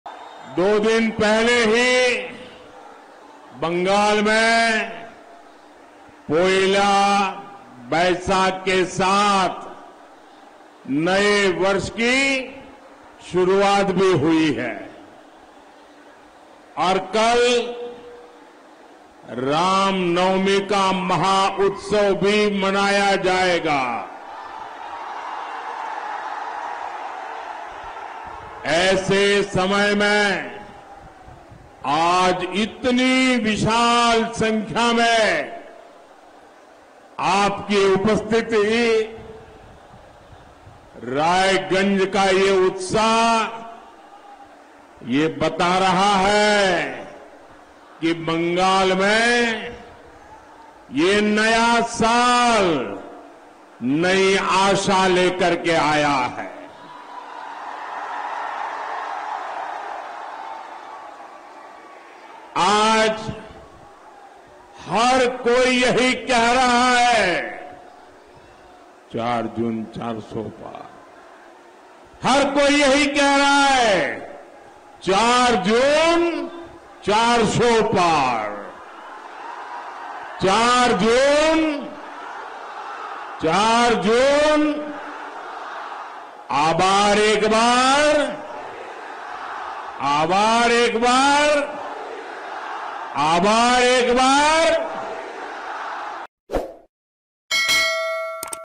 এই বিপুল সংখ্যক উপস্থিতি এবং রায়গঞ্জের মানুষের উৎসাহ বলে দিচ্ছে নতুন বছরে নতুন আশা নিয়ে আসবে
আপনাদের এই বিপুল সংখ্যক উপস্থিতি এবং রায়গঞ্জের মানুষের উৎসাহ বলে দিচ্ছে নতুন বছরে নতুন আশা নিয়ে আসবে।